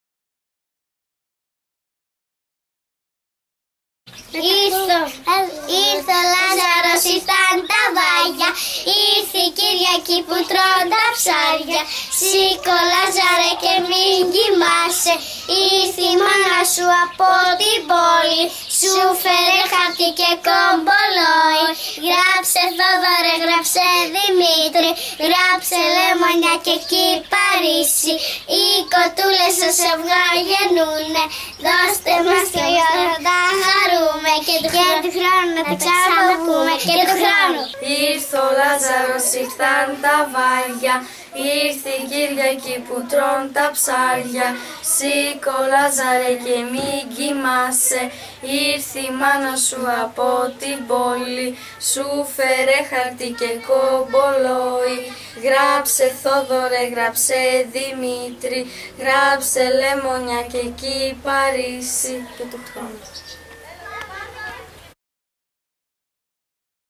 Οι Λαζαρίνες τργουδούν στο Βαλομάνδρι δήμου Πύλης Τρικάλων Σάββατο Λαζάρου 27-4-13
Οι Λαζαρίνες στο Βαλομάνδρι του δήμου Πύλης Τρικάλων Σάββατο του Λαζάρου 27-4-13